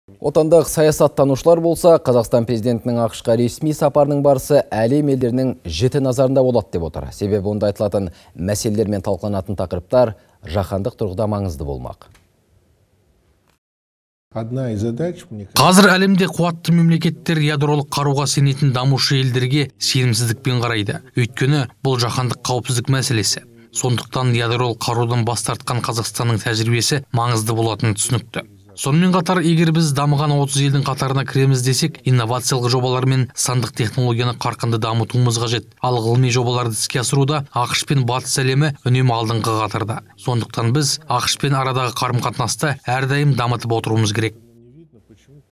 Болат Сұлтанов: Ядролық қарудан бас тартқан Қазақстанның тәжірибесі маңызды
Ал отандық саясаттанушылар: «Қазақстан Президентінің АҚШ-қа ресми сапарының барысы әлем елдерінің жіті назарында болады», – деп отыр.

Себебі онда айтылатын мәселелер мен талқыланатын тақырыптар жаһандық тұрғыда маңызды болмақ.

Болат Сұлтанов, саясаттанушы:

- Қазір әлемде қуатты мемлекеттер ядролық қаруға сенетін дамушы елдерге сенімсіздікпен қарайды. Өйткені бұл – жаһандық қауіпсіздік мәселесі. Сондықтан ядролық қарудан бас тартқан Қазақстанның тәжірибесі маңызды болатыны түсінікті. Сонымен қатар егер біз дамыған 30 елдің қатарына кіреміз десек, инновациялық жобалар мен сандық технологияны қарқынды дамытуымыз қажет. Ал ғылыми жобаларды іске асыруда АҚШ пен Батыс әлемі үнемі алдыңғы қатарда. Сондықтан біз АҚШ-пен арадағы қарым-қатынасты әрдайым дамытып отыруымыз қерек.